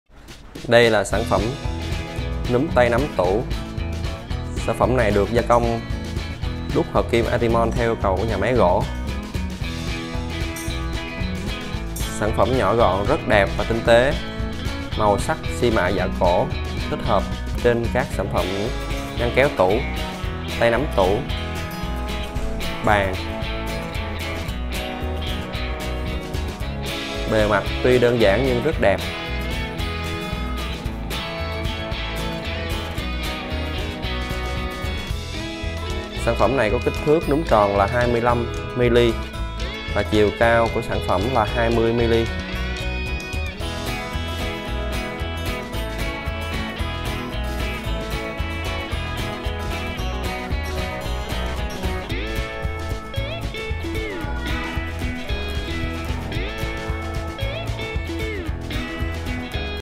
Núm tay nắm tủ giả cổ HD0080
Núm tay nắm tủ HD0080 xi mạ đồng giả cổ 25x25mm. Thiết kế đơn giản, chất liệu Atimon nguyên khối xi mạ giả cổ (hoặc màu theo yêu cầu khách hàng).
Thông tin kỹ thuật
- Chất liệu: Atimon
- Màu sắc: xi mạ giả cổ (đồng hoặc theo yêu cầu khách hàng)
- Kích thước: đường kính núm tròn 25mm, chiều cao 25mm
HD0080 cabinet knob. Antimony antique-style for cabinet doors, cabinet drawer. Products with high quality, durable color.
Product information
- Material: antimony
- Color: antique silver (or color according to customer requirements)
- Dimensions: 25mm